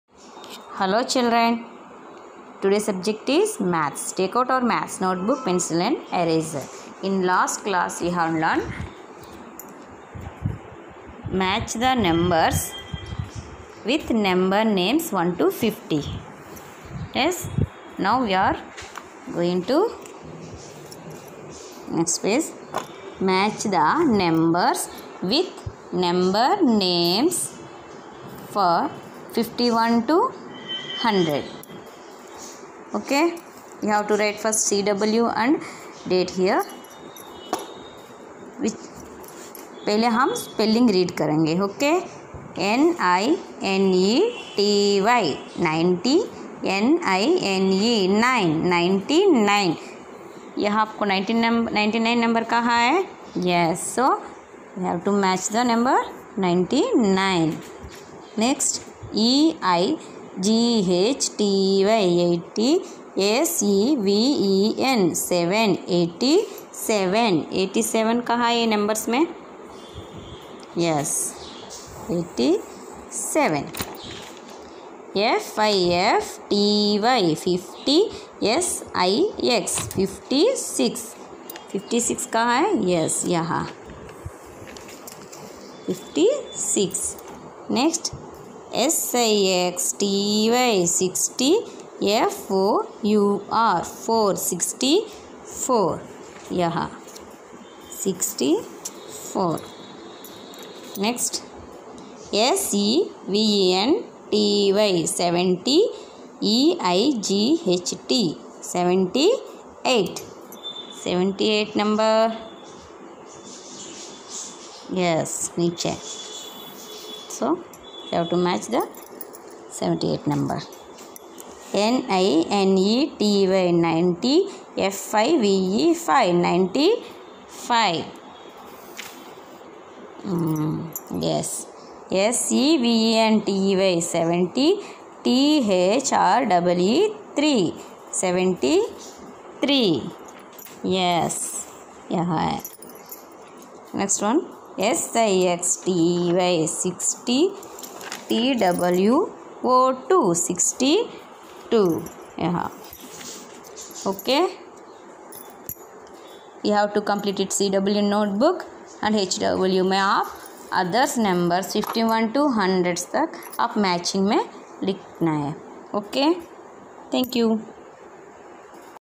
Senior KG - Maths - Revision - Number names matching - 51 to 100
Video from Vaishali